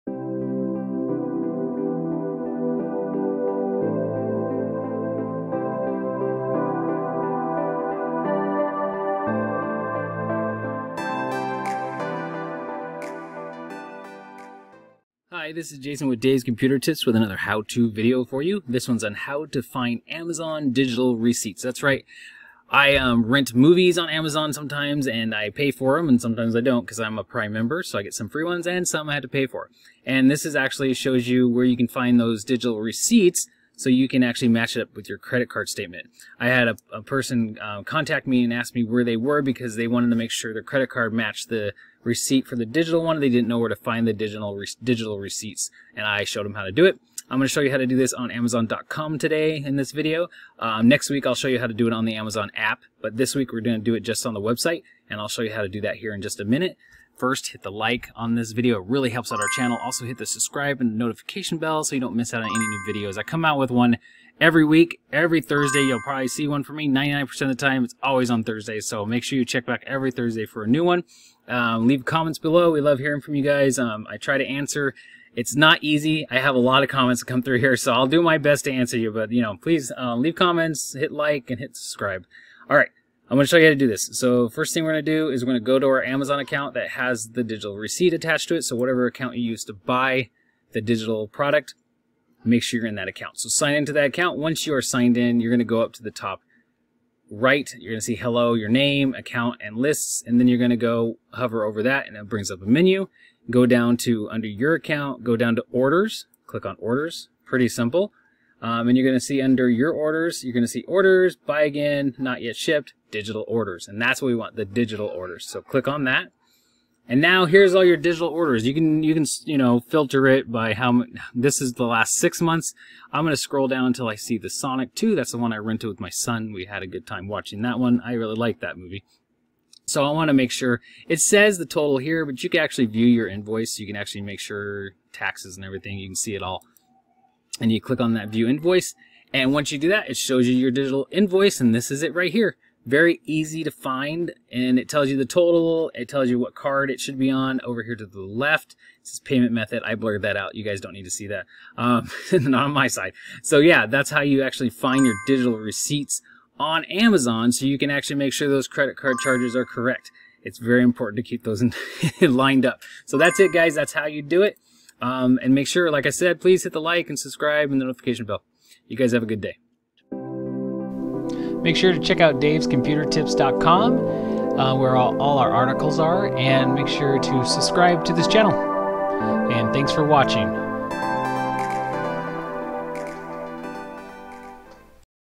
How To Find Amazon Digital Receipts
In this video, I will show you how to find those Amazon digital receipts.
On the DCT YouTube channel, we receive many questions. Recently, I was asked a question on one of our Amazon ‘How To’ videos. The person asked how they could find the amount they paid for a digital purchase. They wanted to make sure that the charge was correct on their card. This is a question that I’m asked often so why not show all of you how to access this information? Before we move forward, here are some other videos that I have created about Amazon:

Now that you have all that knowledge, let’s dive into this new lesson. There might be many reasons you want to find this digital receipt but for whatever reason it is, we will be showing you the steps to retrieve it. Open that Amazon account and let us get this started.